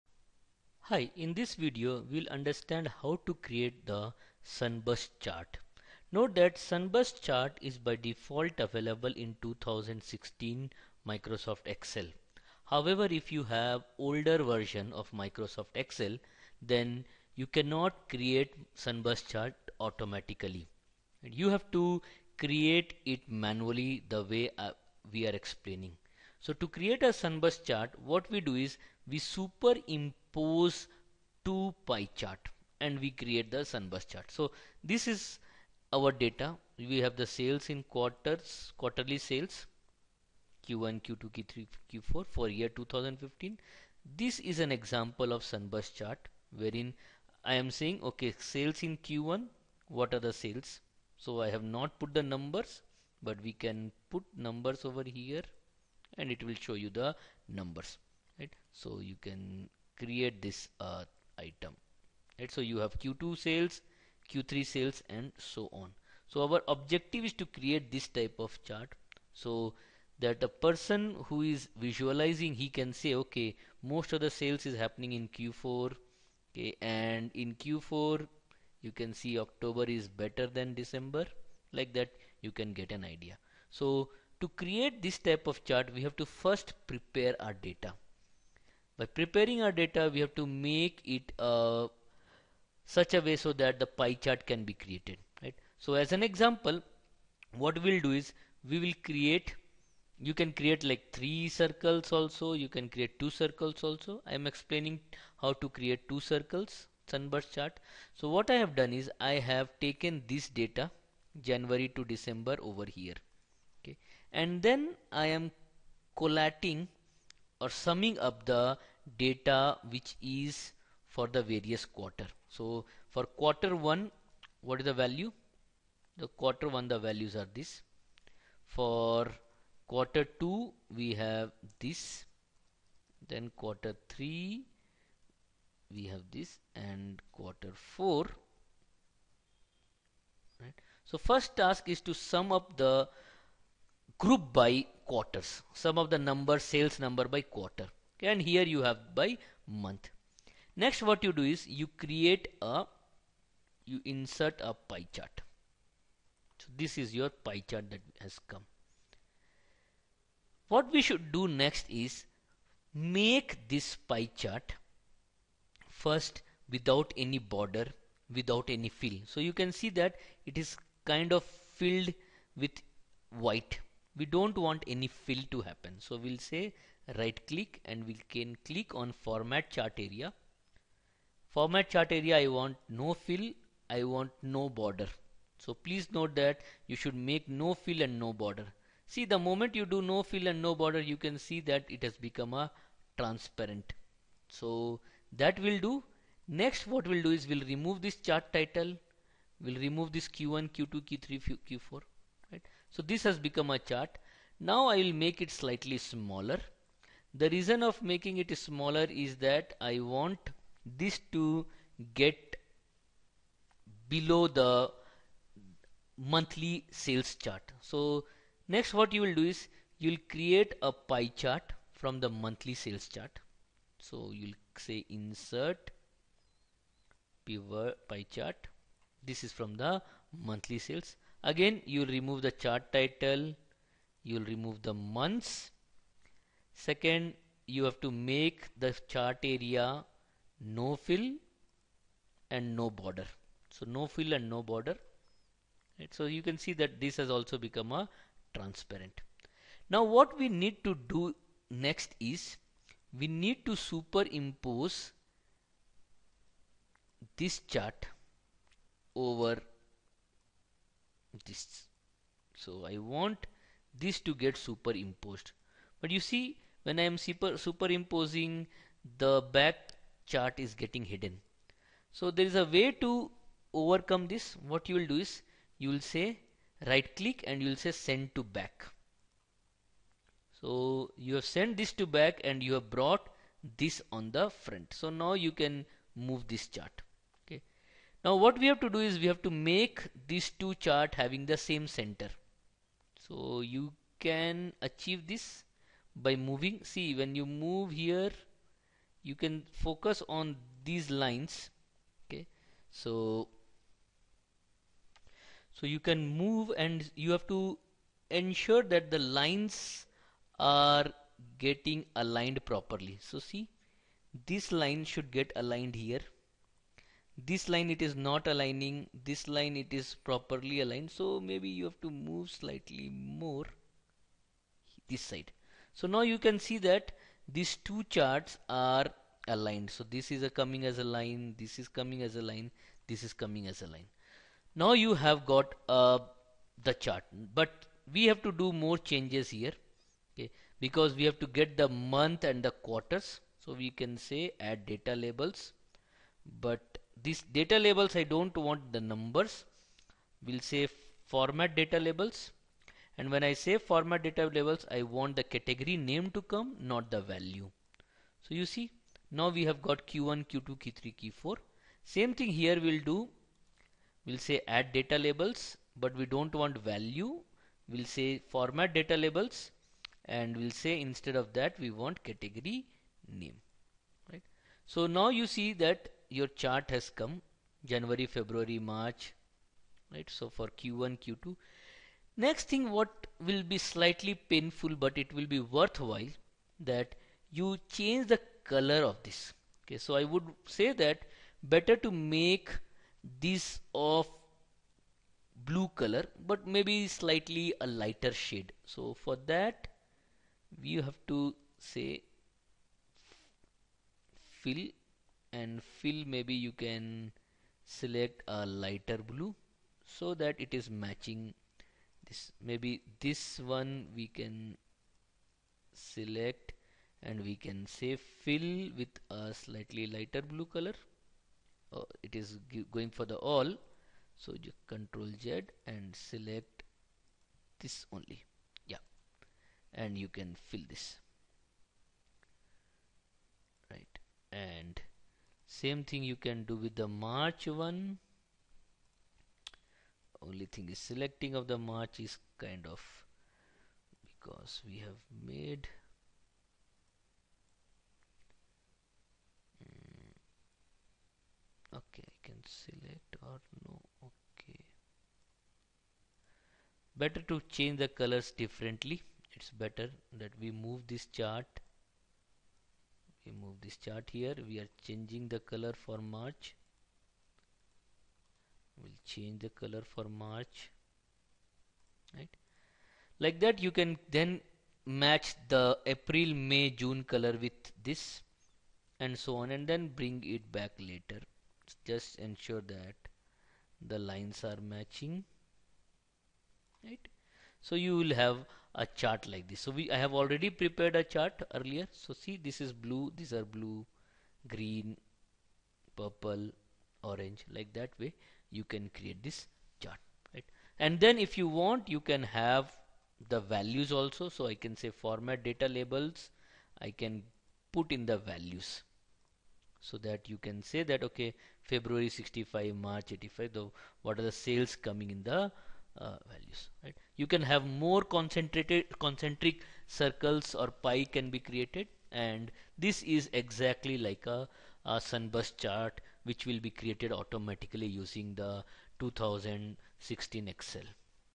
How to create Sunburst Chart in Excel
A Sunburst Diagram is an easy-to-interpret and amazingly insightful visualization. You should give it a try in your data stories before the year elapses. We understand Excel is the go-to data visualization tool, especially among small and medium-sized businesses (SMBs). But, the application comes with a pretty basic Sunburst Chart. You need extra effort and time to customize the chart to align with your requirements. Excel Sunburst Chart is a built-in chart available in Excel 2016 that is used to display a hierarchical structure data in circular form. Just like a doughnut chart, Sunburst Chart is also used to display a part of the whole data and compare relative sizes. But it can also show the relationships in the hierarchy. Sunburst Chart also known as the Sunburst Partition Chart, Ring Chart, or Radial Tree map, displays hierarchy in your raw data. Sunburst Diagram displays hierarchal insights through a series of concentric rings. Each circle in the Sunburst Chart corresponds to a level in the hierarchy of data points. More so, each ring is segmented proportionally to represent its constituent details. In this video we show how to create Sunburst charts in older versions of Excel. Excel 2016 has sunburst chart as inbuilt functionality.